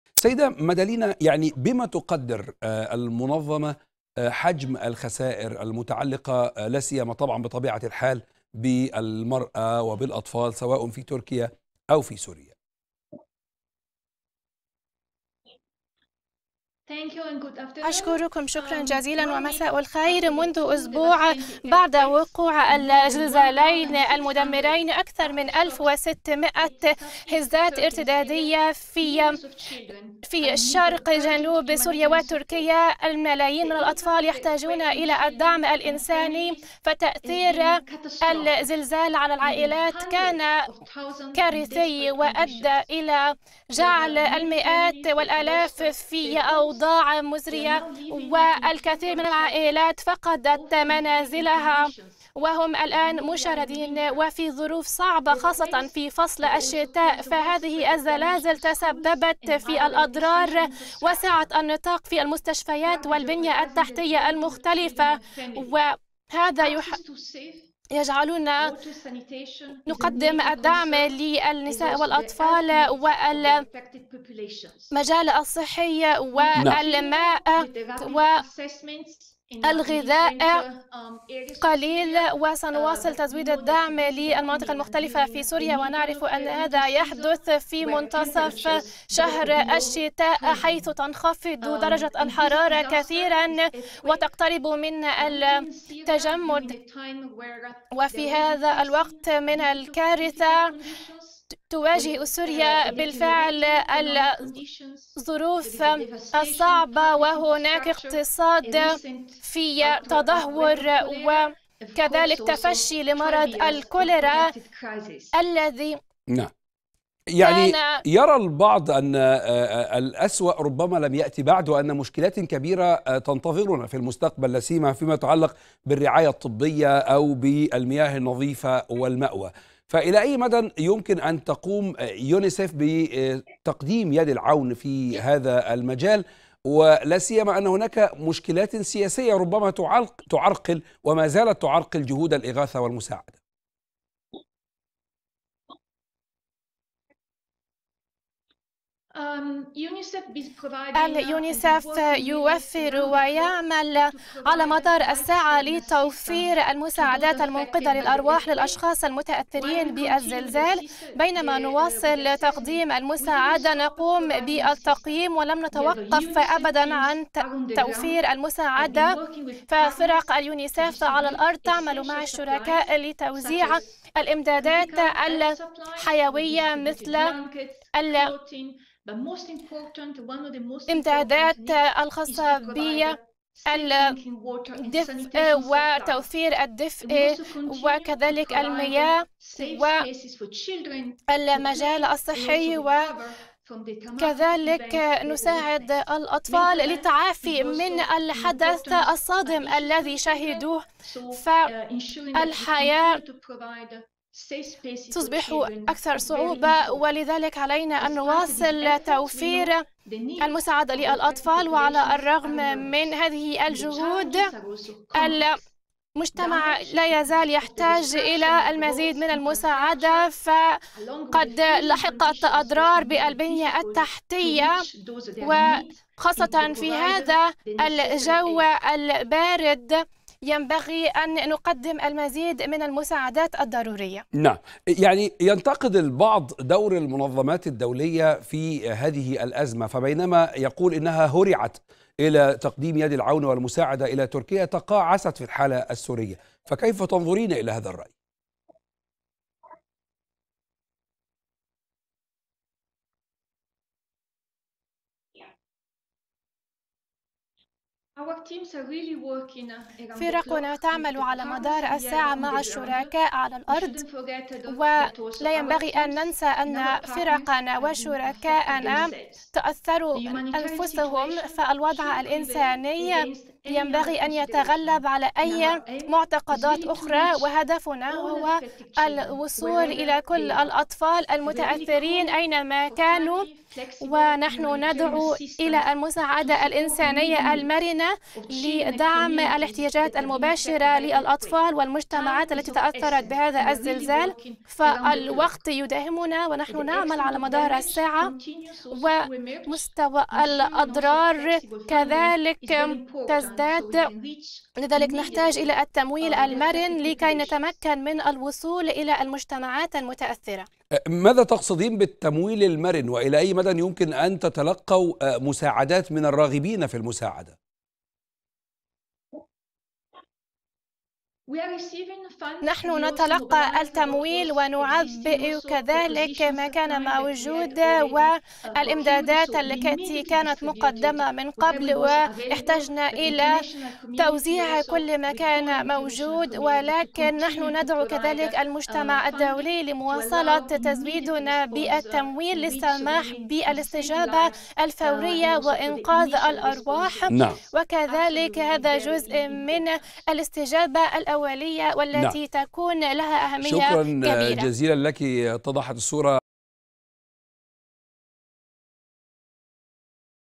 اليونيسيف للغد: هدفنا هو الوصول لكل الأطفال المتضررين من الزلزال.. أينما وجدوا
تواصل فرقُ إغاثة من عشرات الدول عملياتِ البحث عن ناجين وسط الأنقاض، ورفعَ ركام المباني المنهارة. ورغم تضاؤل فرص العثور على ناجين مع مرور الوقت، شهدت الساعات الماضية إنقاذ بعض الحالات بعد أكثر من مائتي ساعة تحت الأنقاض. المزيد من التفاصيل مع مادالينا بيرتيروتي نائب المدير الإقليمي لليونيسيف بشمال أفريقيا والشرق الأوسط ومديرة البرامج والتخطيط عبر النشرة الإخبارية مع الإعلامي حسن فودة.
*تغطية إخبارية لأحداث زلزال تركيا وسوريا